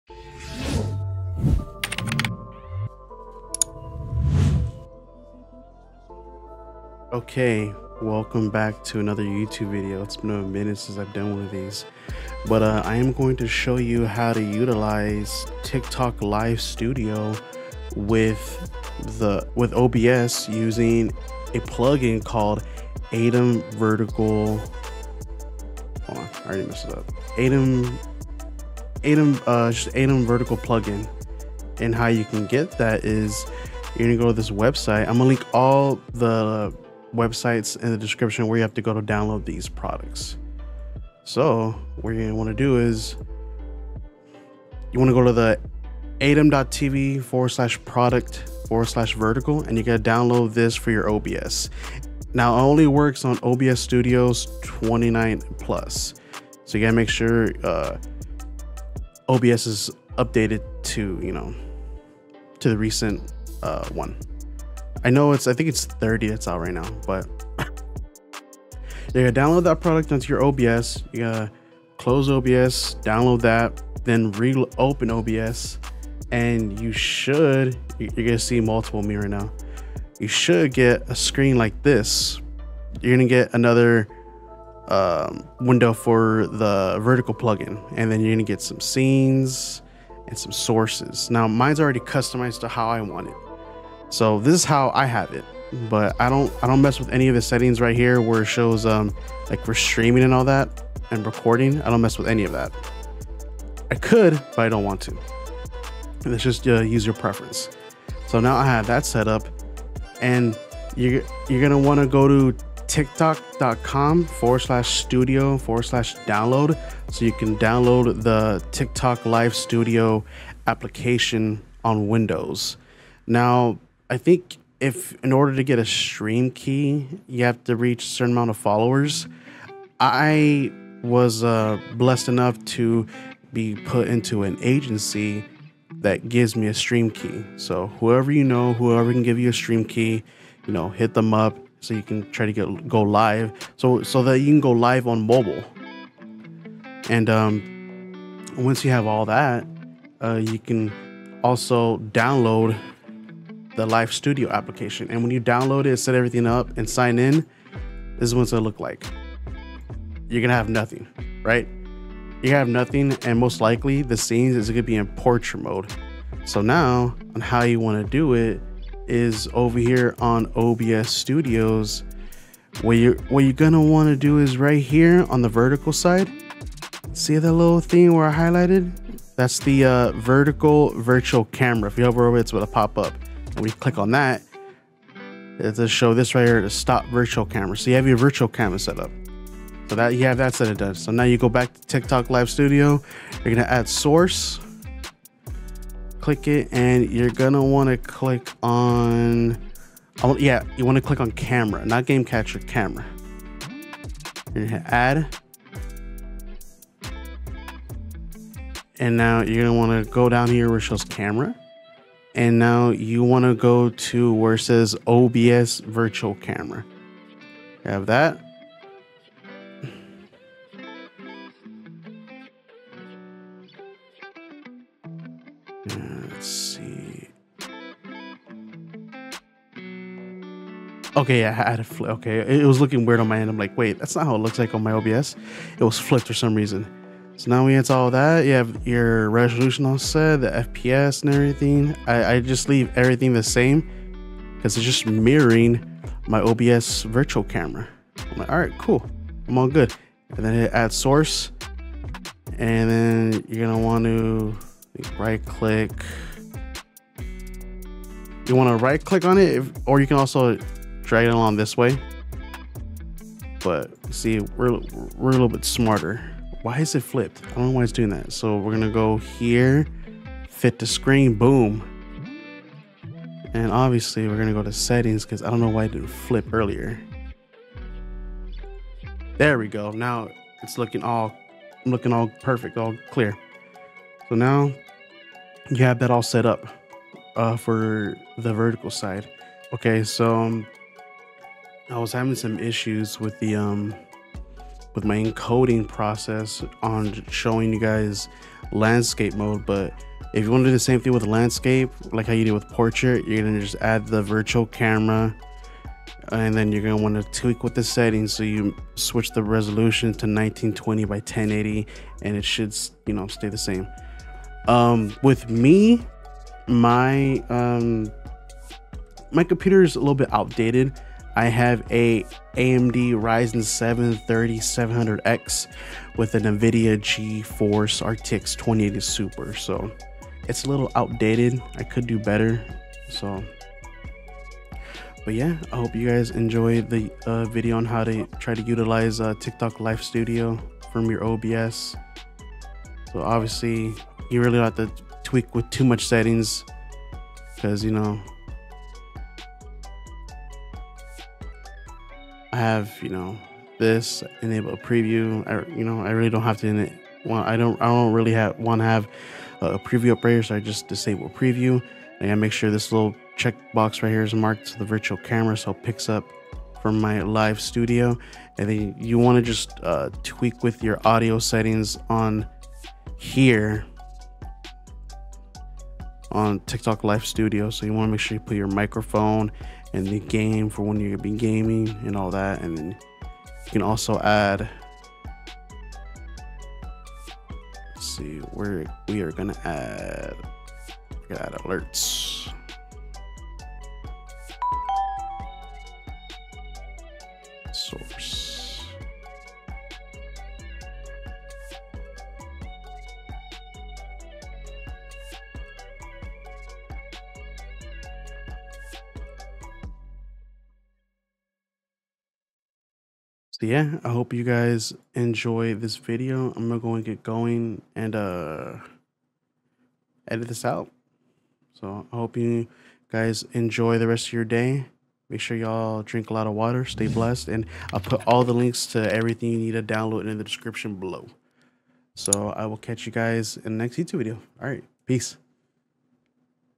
HOW TO SETUP TIKTOK LIVE STUDIO USING AITUM VERTICAL PLUGIN (OBS Studio) AUGUST 2024
Here's a video on how to setup your Aitum Vertical Plugin (OBS Studios) with TikTok Live Studio. If you have any questions please let me know below.